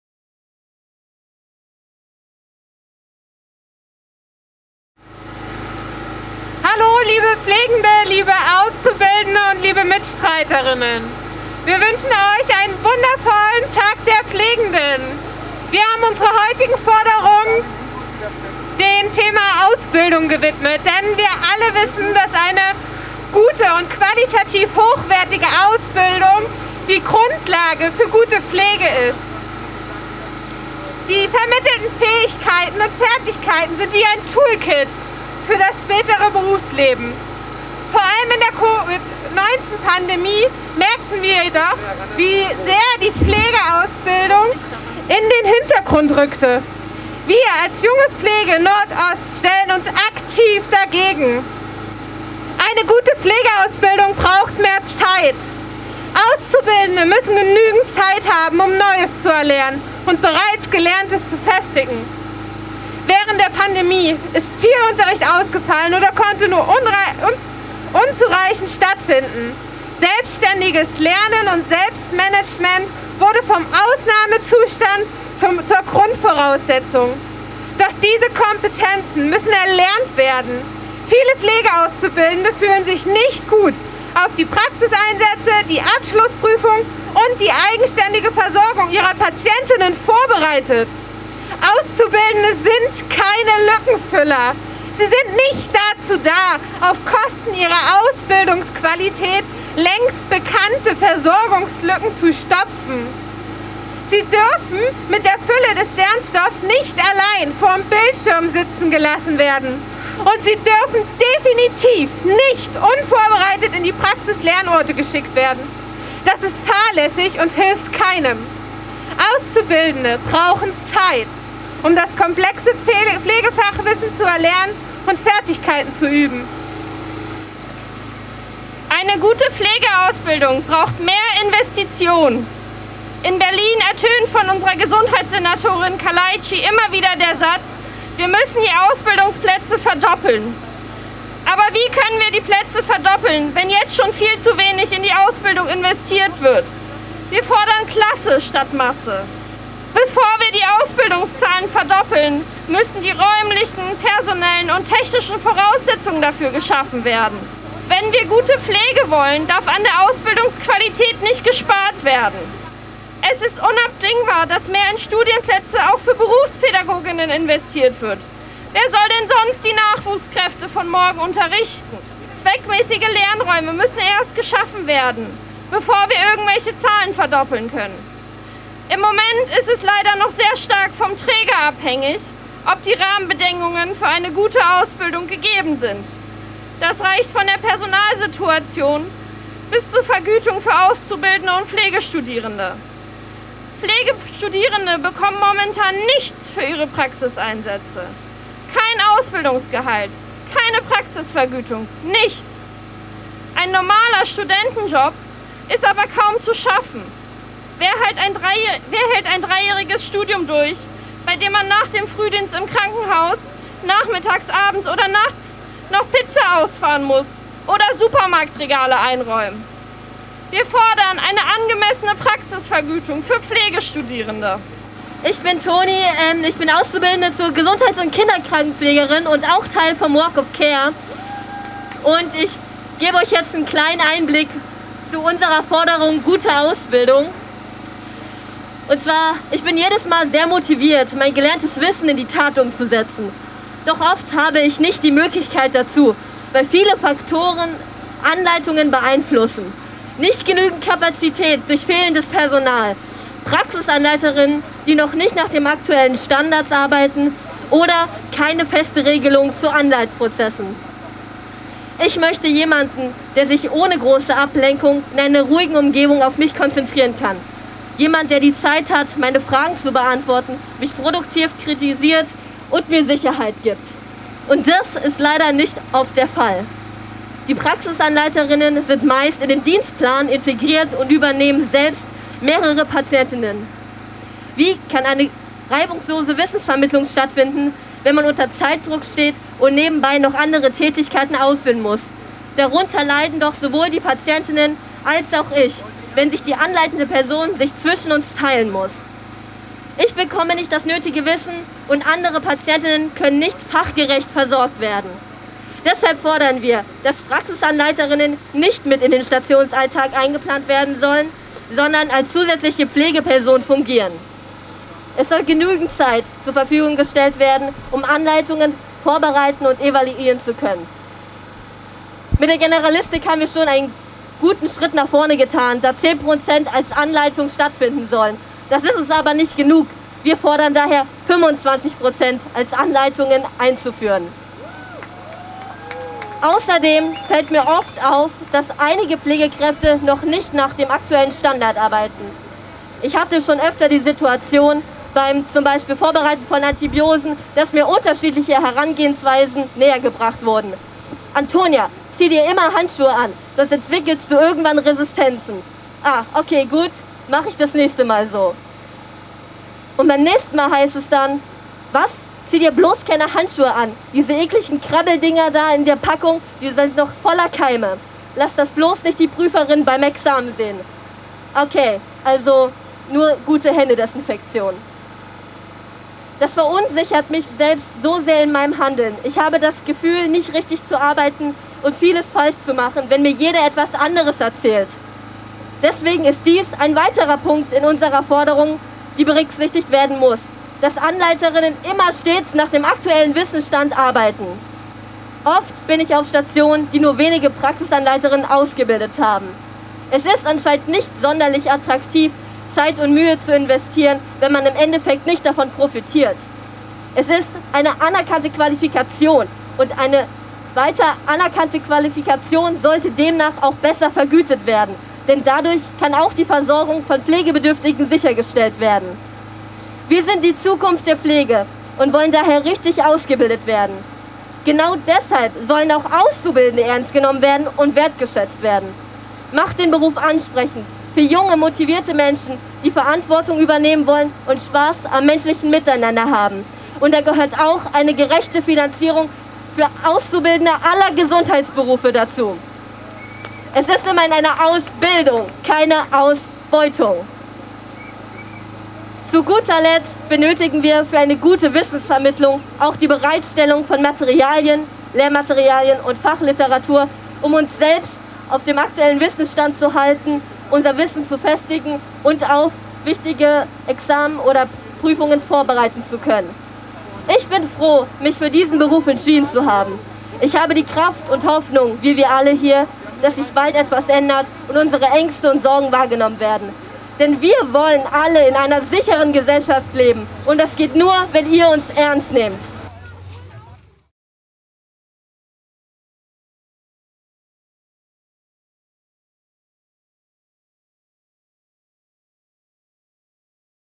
EU Schwerbehinderung Walk of Care Berlin 20210512
Im Jahr 2021 steht der "Walk of Care" in Berlin im Zeichen der Ausbildung um so auf die Probleme in der Pflege-Ausbildung aufmerksam zu machen